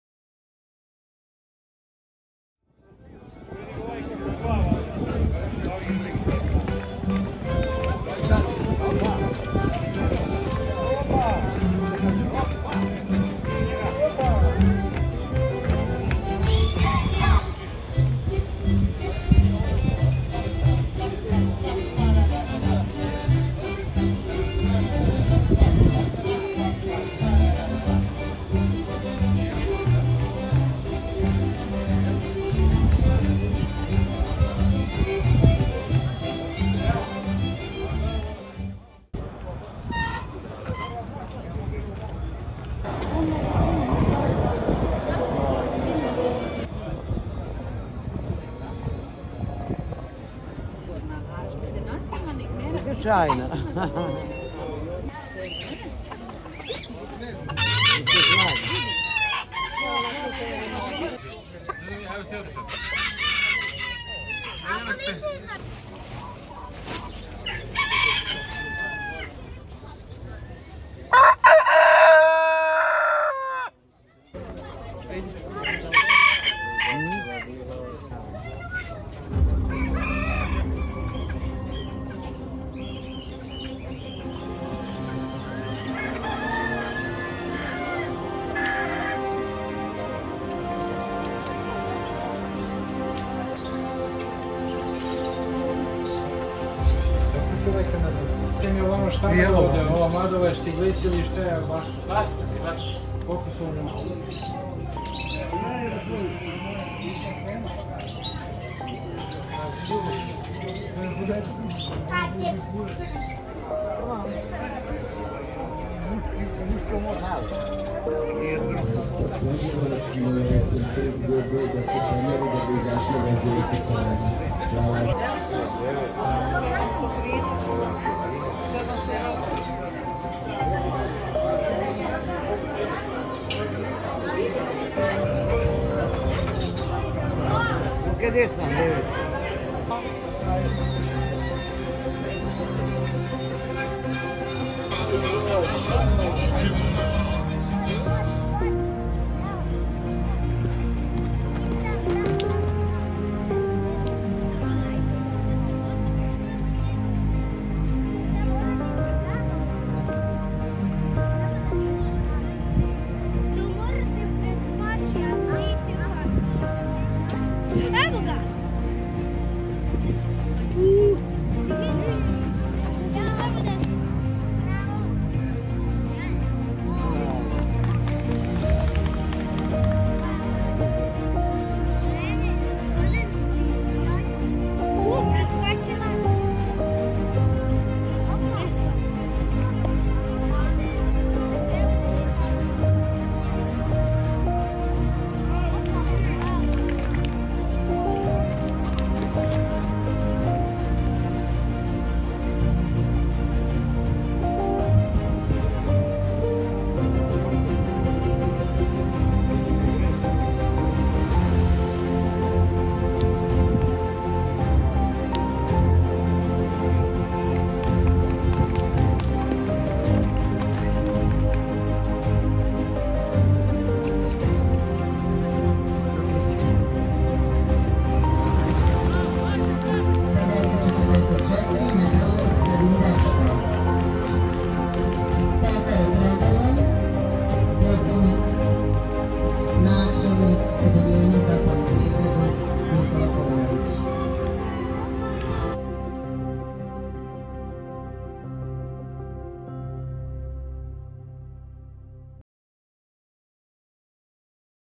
Agriculteral show in Kragujevac Serbia 24
Me and Svetlana came back to Kragujevac in Serbia, this show was on for 3 days. we went on first day. These aresome of events we saw.